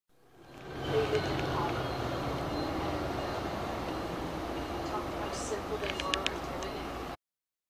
VIA Rail 639
Another VIA Rail train, 639, caught from another view.